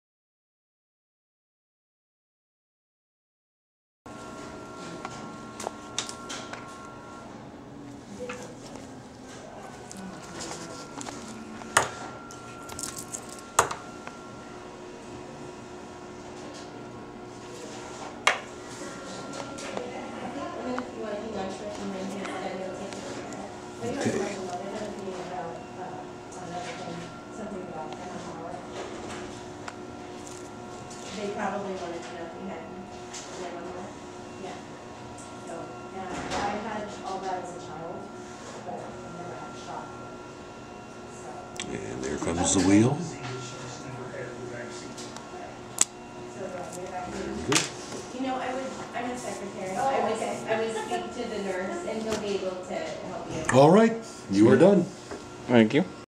Tuberculosis (TB) Test
This is me getting a TB test. Please don't watch if you're scared of needles, or people.